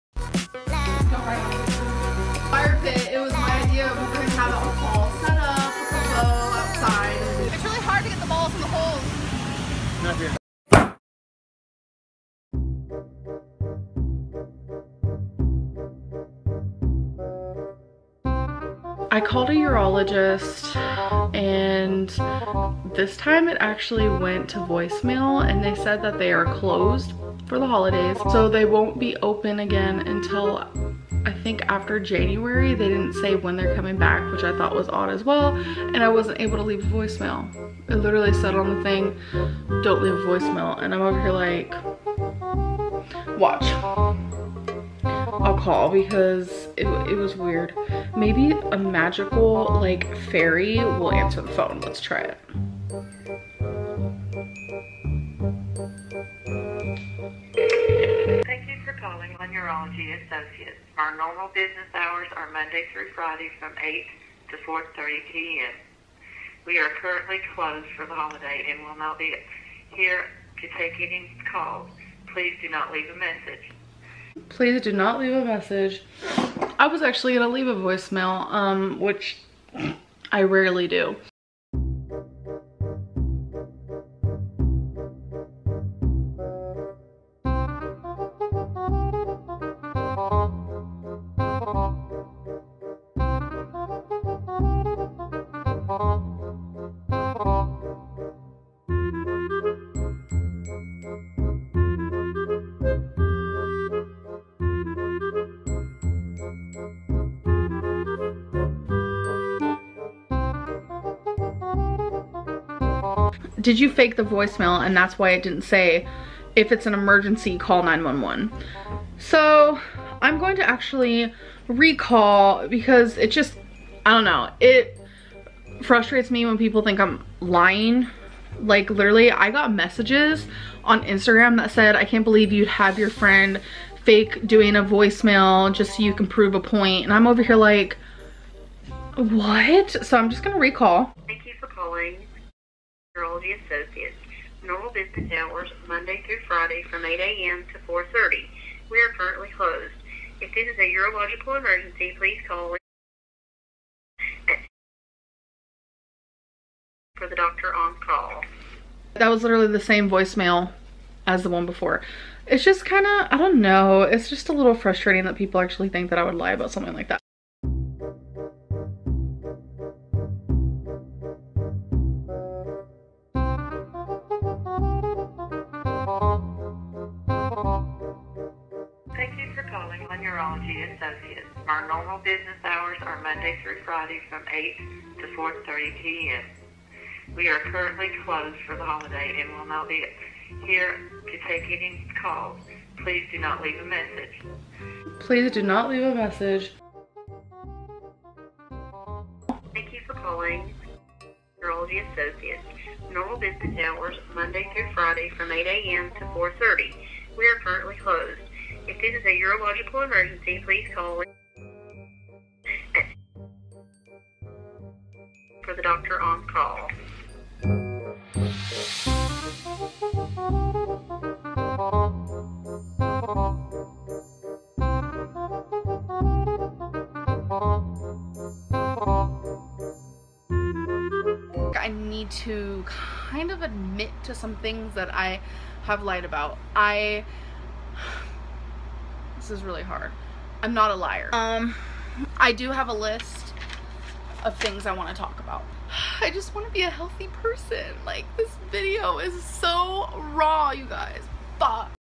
Amberlynn and her urologist drama
our gorl is in another drama yet again. comment if you think she faked the voicemail